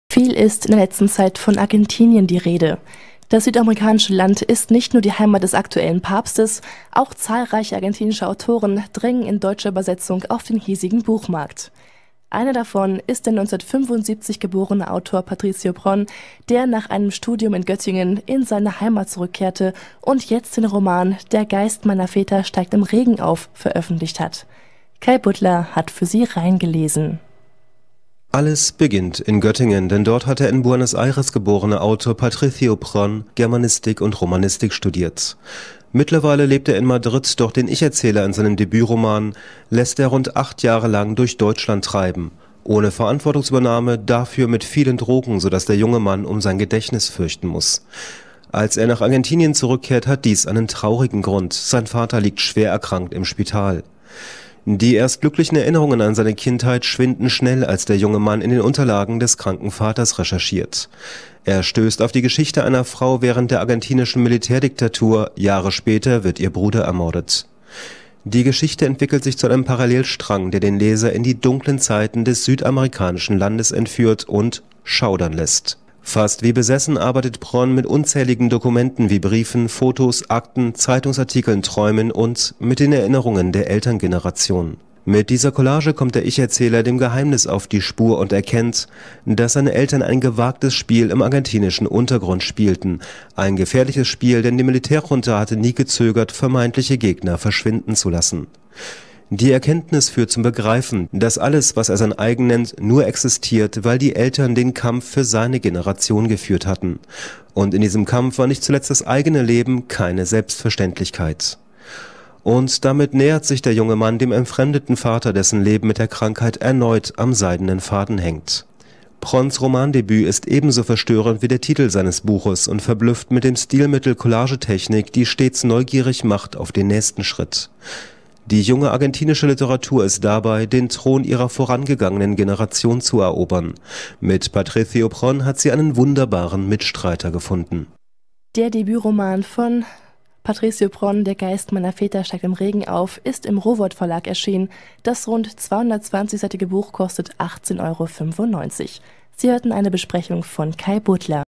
Patricio Pron - Kai Budlers Rezension des Romanes "Der Geist meiner Eltern steigt im Regen auf"
StadtRadio Göttingen, 26 September 2013.